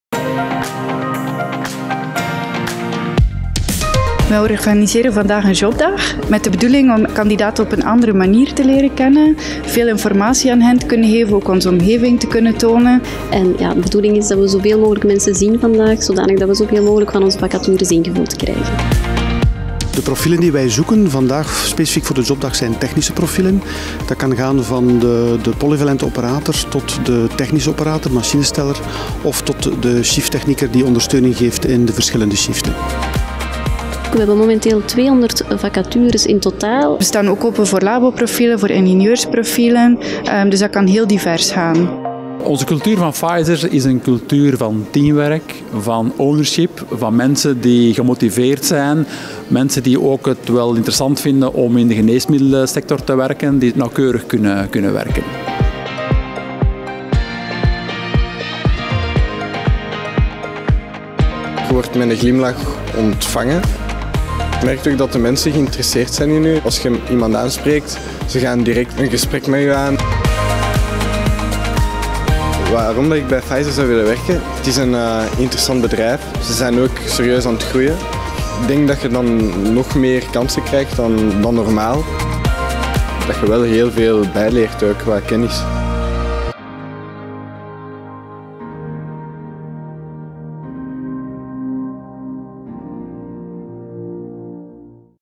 Pfizer Puurs organiseert jobdag op 21 april 2018
Onze site in Puurs is één van de grootste productiesites van Pfizer in de wereld. De hoogtechnologische installaties draaiende houden, vraagt heel wat precisiewerk. Na onze jobdag in november verwelkomden we 34 nieuwe techniekers en technisch operatoren. En onze zoektocht gaat verder. Heb jij talent voor techniek? Schrijf je dan nu in voor een nieuwe editie van onze jobdag op 21/4 en ontdek onze unieke jobs.